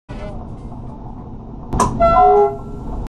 Horn eines See GTW der Thurbo
Leider ein sehr sehr kurzes Video aber ich lade es trotzdem hoch weil es ein MAKRO ist und das wollt ihr ja sehen auf meinem Channel ;-D

Nächstens kommt noch ein tolles Führerstandsvideo hoch das mit der GoPro Hero+ in 1080p 60 fps aufgenommen wurde... bleibt gespannt...